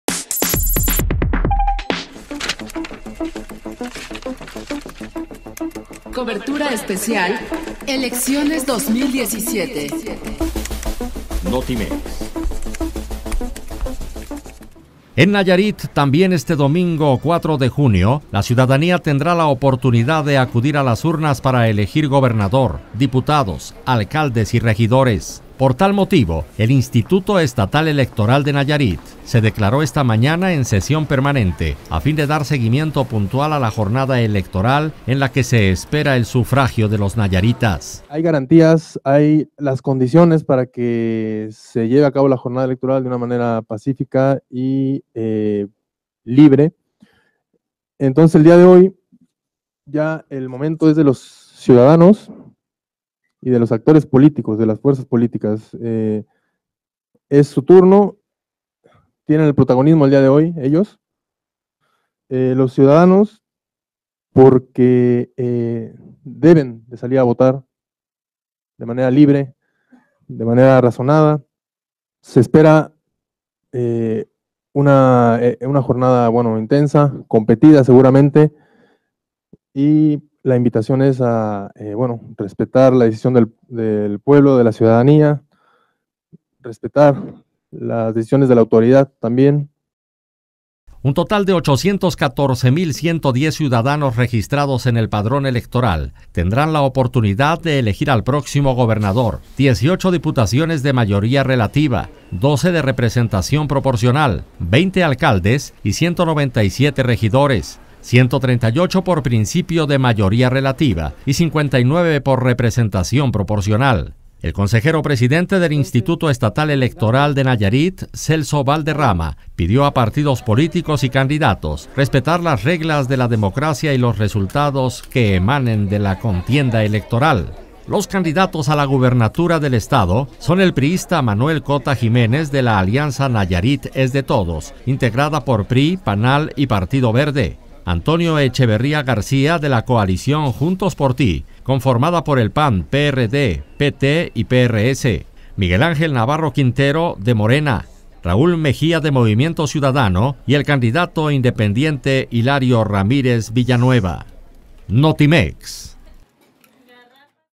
El Instituto Estatal Electoral de Nayarit llama a civilidad y respetar resultados
El consejero presidente del Instituto Estatal Electoral de Nayarit, Celso Valderrama demandó a partidos y candidatos a respetar las reglas de la democracia y los resultados de los comicios